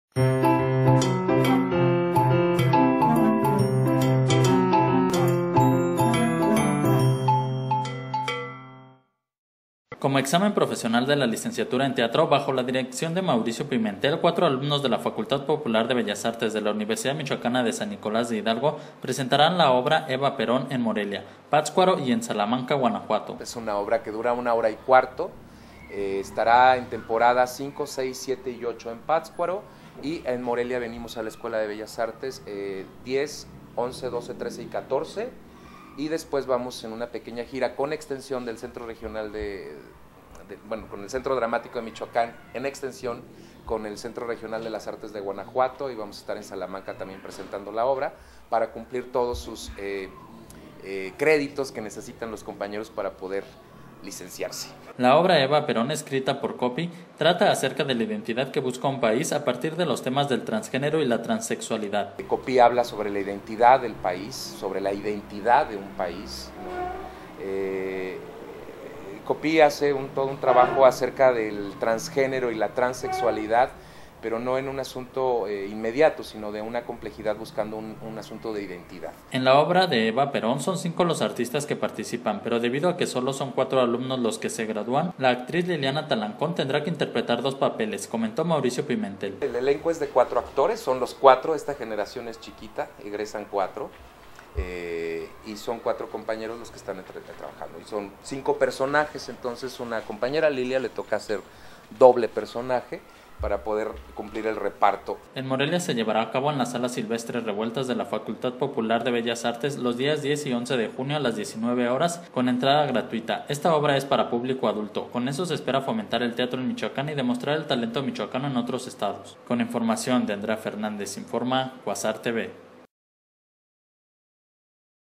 Alumnos de Bellas Artes montarán obra ''Eva Perón''
Como examen profesional de la Licenciatura en Teatro, bajo la dirección de Mauricio Pimentel, cuatro alumnos de la UMSNH presentarán la obra "Eva perón", en Morelia, Pátzcuaro y en Salamanca, Guanajuato...